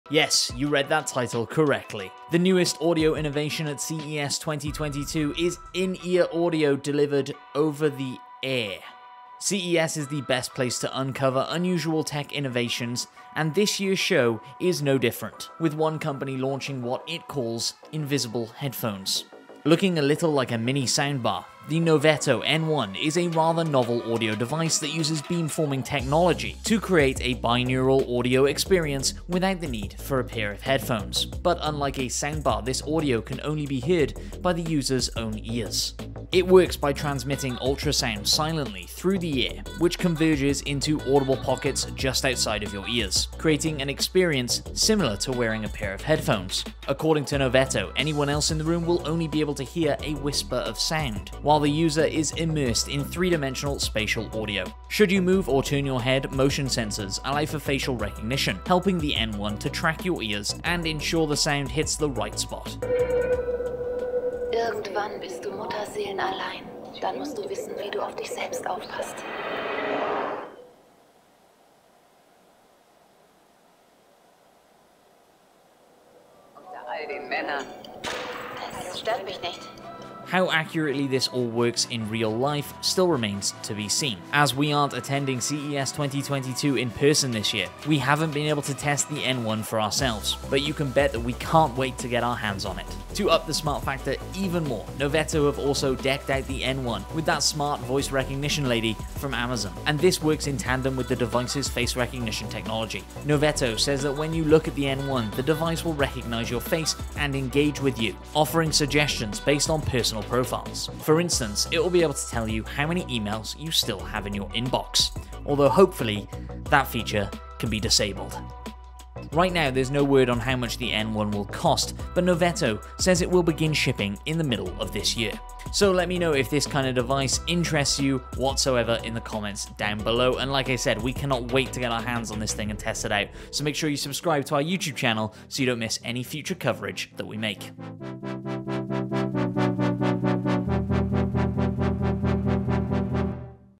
Invisible headphones are as weird and awesome as they sound
CES is the best place to uncover unusual tech innovations, and this year's show is no different, with one company launching what it calls a pair of "invisible" headphones.

Looking a little like a mini soundbar, the Noveto N1 is a rather novel audio device that uses beamforming technology to create a binaural audio experience without the need for a pair of headphones – but unlike a soundbar, this audio can only be heard by the user's own ears.

It works by transmitting ultrasound silently through the air, which converges into audible pockets just outside of your ears - creating an experience similar to wearing a pair of headphones.